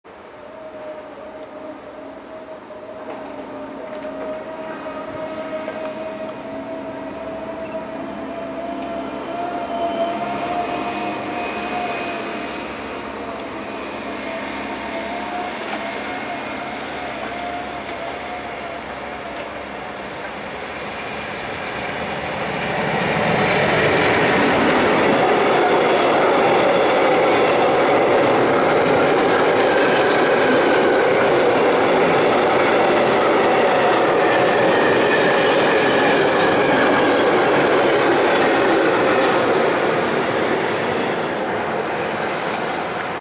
142 130-4 in Vallendar
Heute konnte ich die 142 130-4 mit dem bekannten Henkelzug in Vallendar filmen.